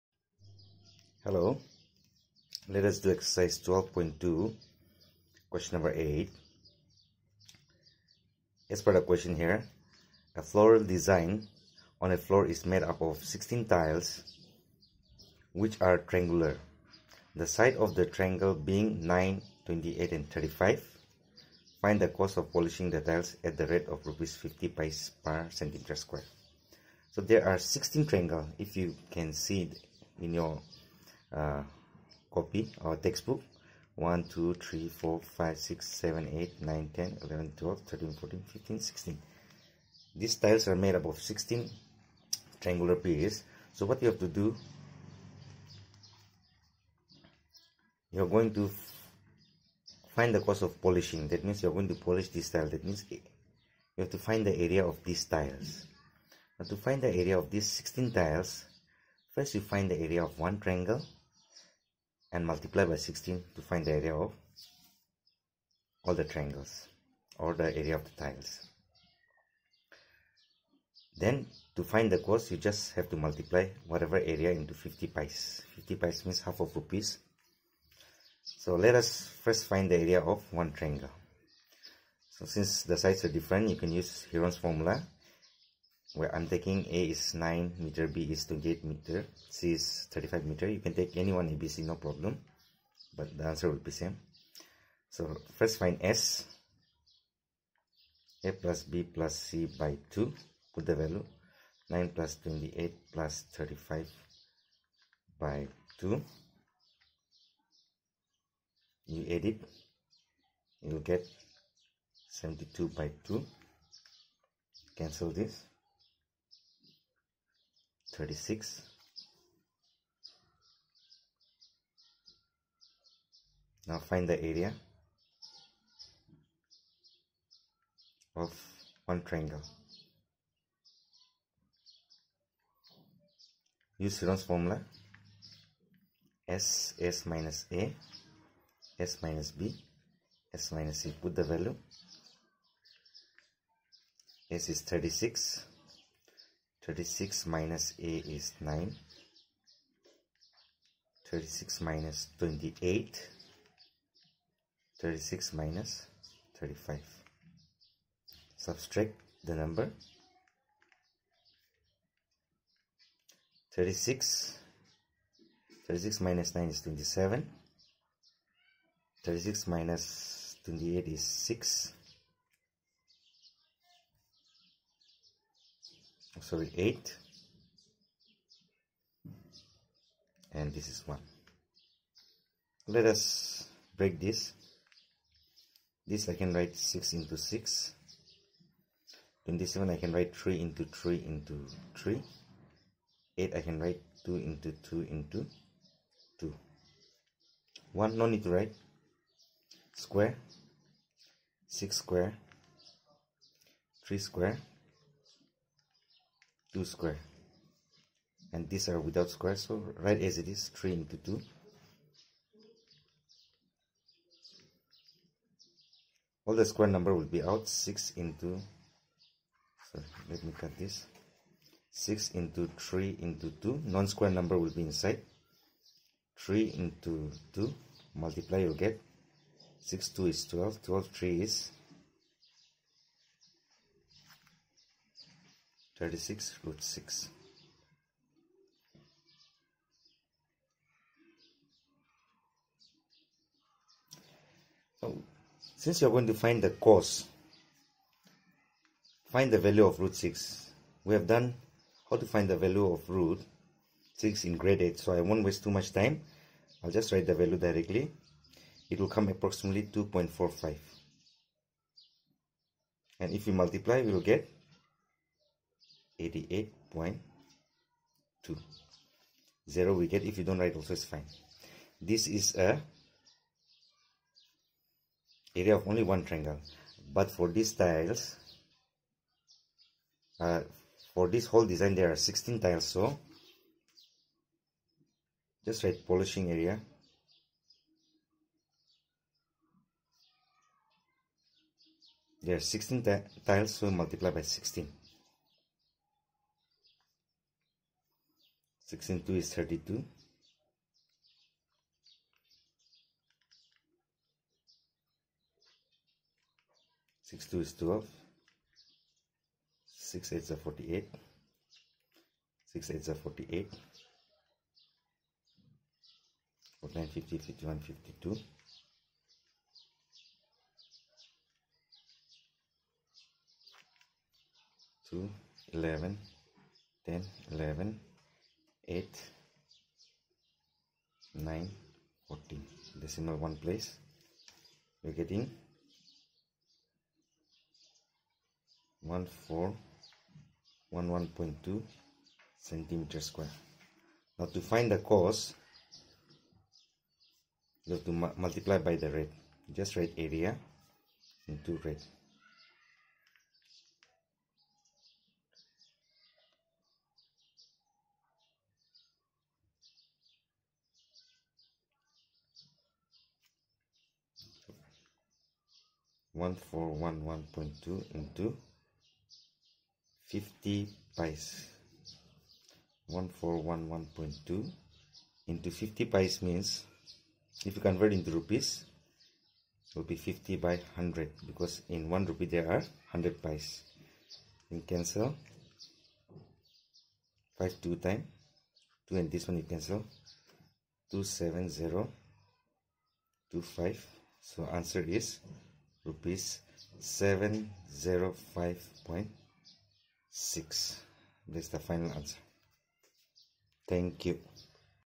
Ncert Math Class 9 Chapter 12.2 QNo. 8 in English
Heron's Formula Ncert Math Class 9 Chapter 12.2 QNo. 8 in English solved by SKOOL FOR ALL